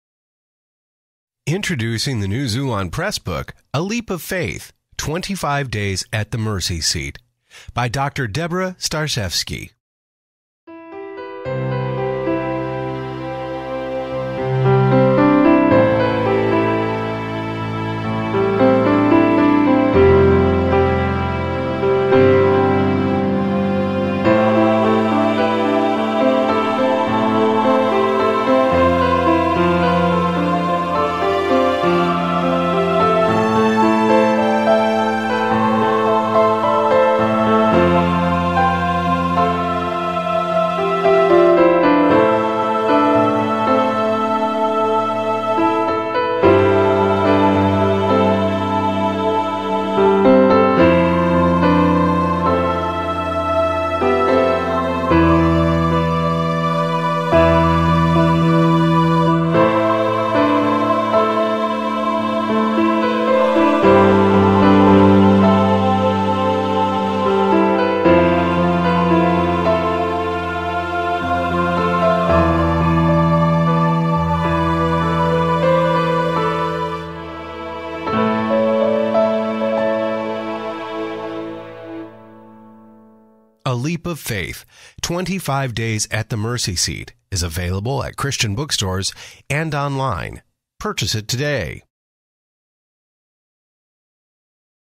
Xulon Press book A LEAP OF FAITH | DR DEBORAH STARCZEWSKI
Xulon Press introducesA LEAP OF FAITH by DR DEBORAH STARCZEWSKI. The Xulon Press bookstore provides an easy way to purchase this book using the link below, or you can find it at your local Christian bookstore.: